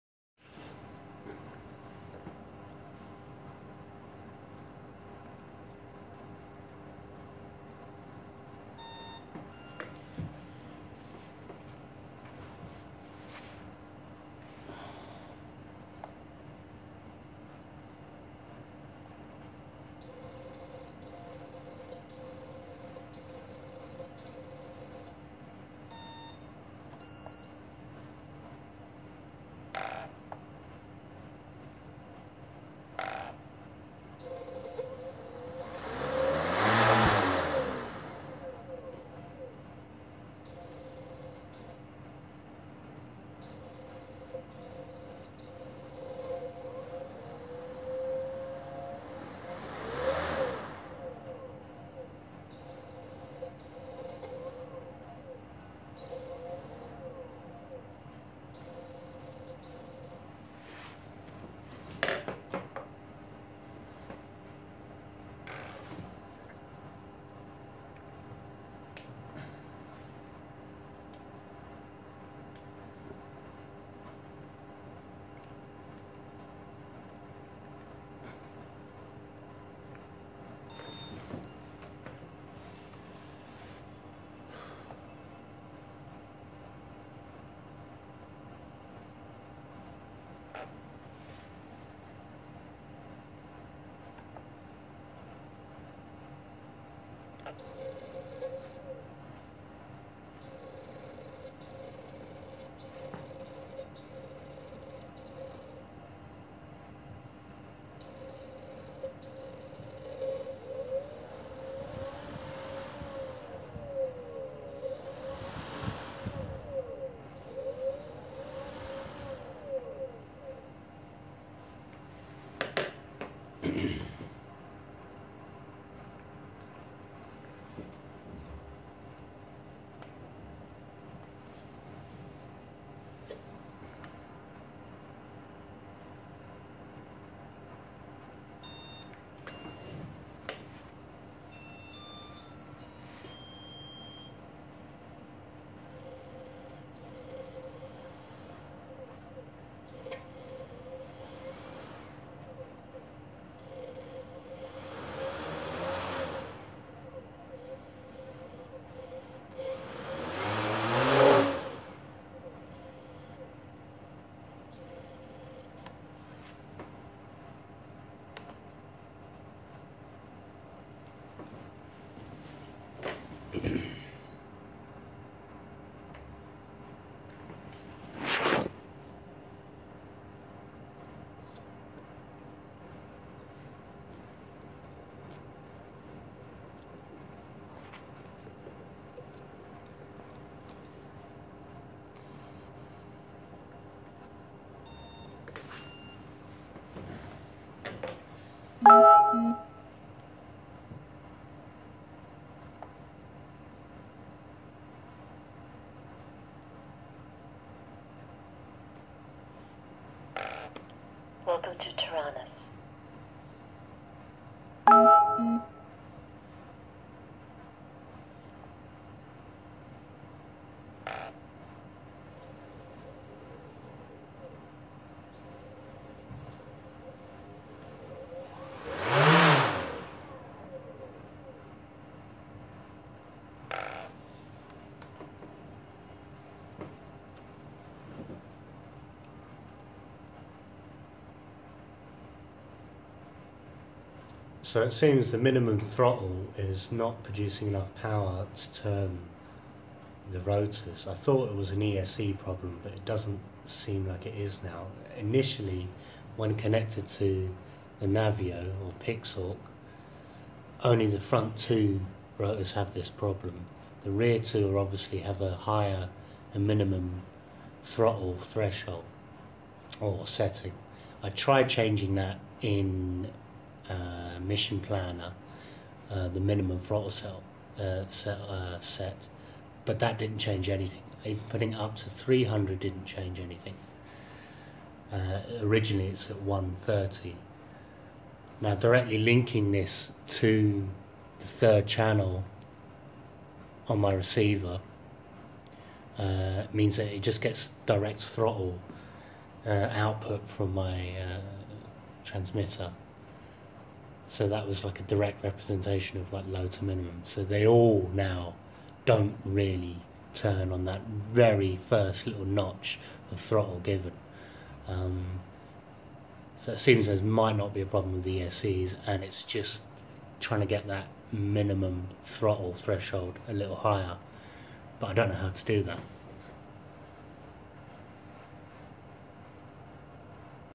Motor stutter. Diagnostics. Sunnysky 2212. 20a blheli esc. Navio2.
Having problems with low throttle motor stutter. Thought it was esc's. Now testing using receiver channel 3 output to individual esc's (direct connection to transceiver throttle) and all motors show some stutter as minimum throttle. The original front two motors that had the problem have a very slightly higher threshold.